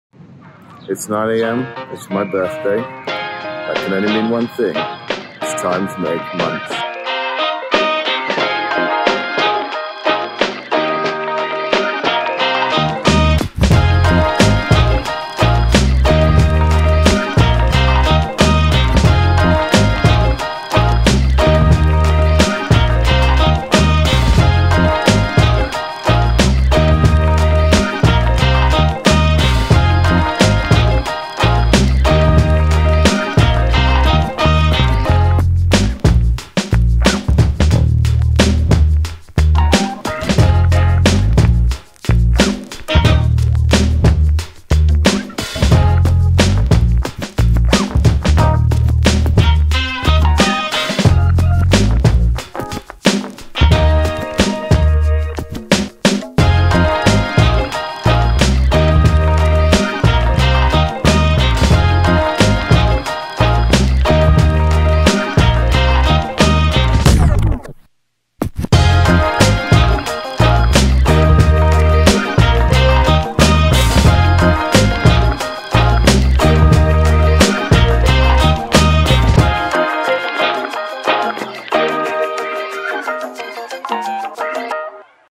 Birthday Pulled Pork
I decided to barbecue a 4kg pork shoulder on the bone for my birthday earlier on this year. Great fun day with my wife and friends. Shame I forgot to get a shot of pulling the pork.